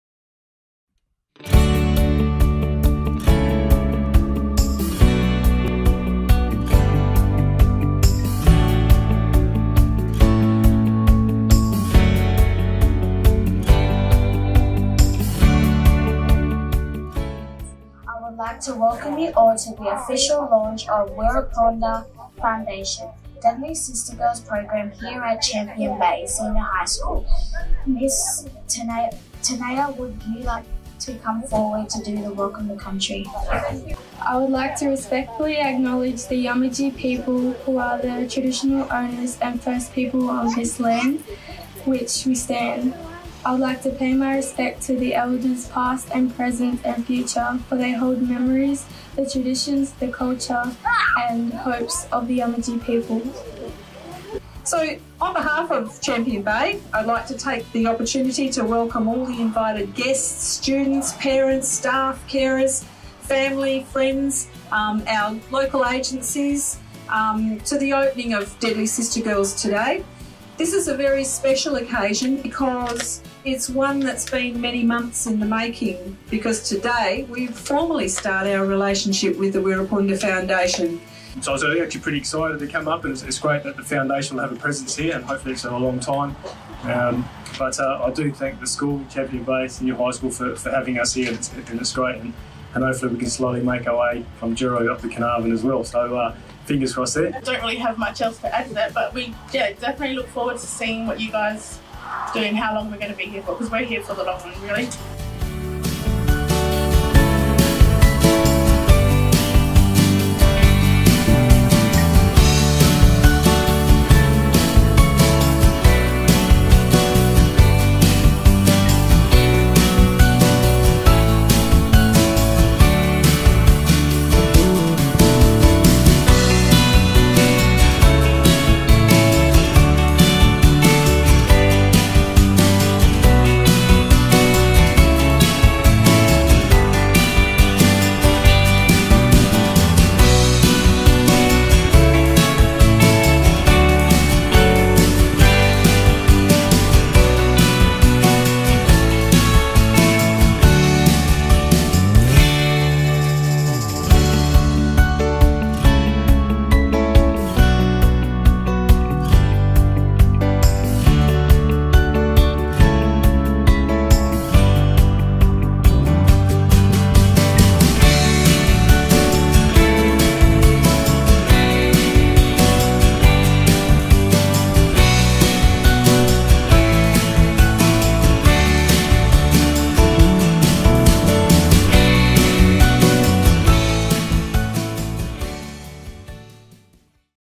Deadly Sista Girlz Geraldton Launch
We headed to Geraldton the launch our Deadly Sista Girlz Program at Champion Bay Senior High School.

Delivered by strong Aboriginal and Torres Strait Islander role models, Deadly Sista Girlz is a culturally appropriate healthy lifestyle program which aims to improve engagement and education among school-aged Aboriginal and Torres Strait Islander girls.